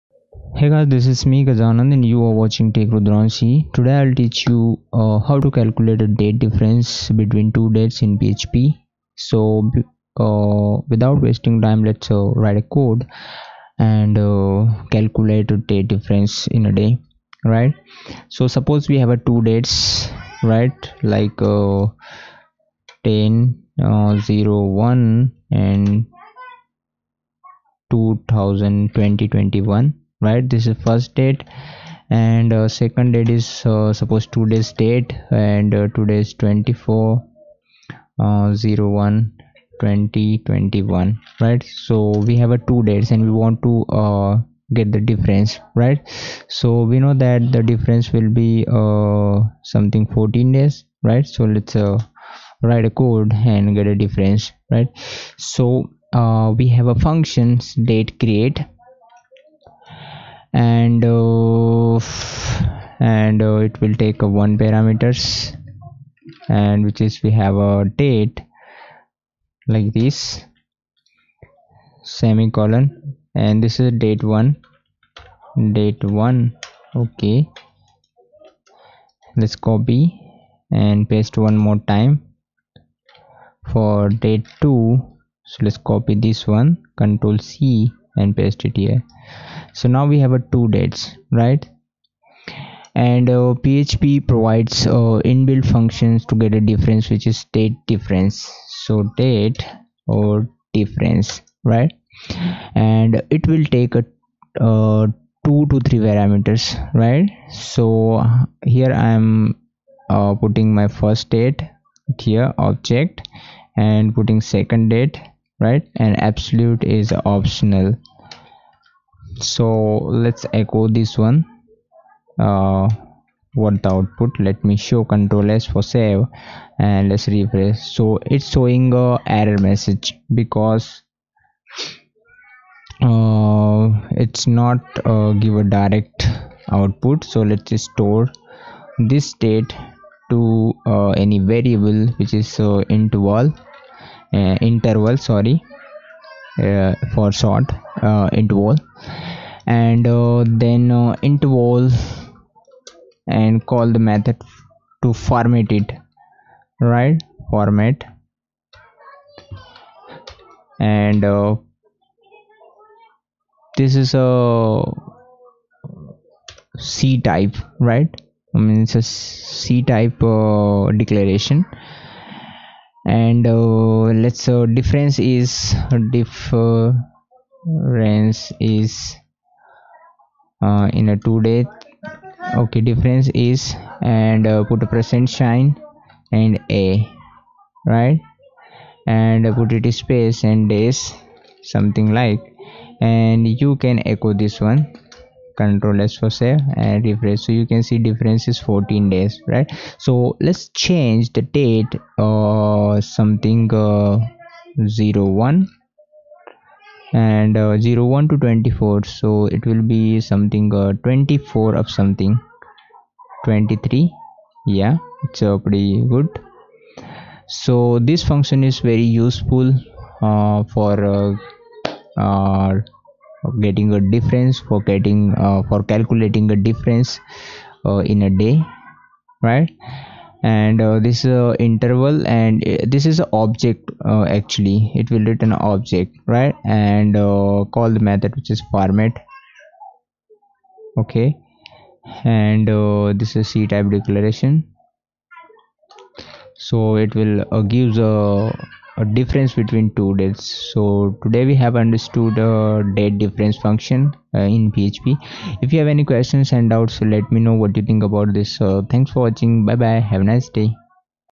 Calculate Date Difference and Get Days in PHP
In this tutorial, we will learn how to calculate the difference between two dates and get the remaining days.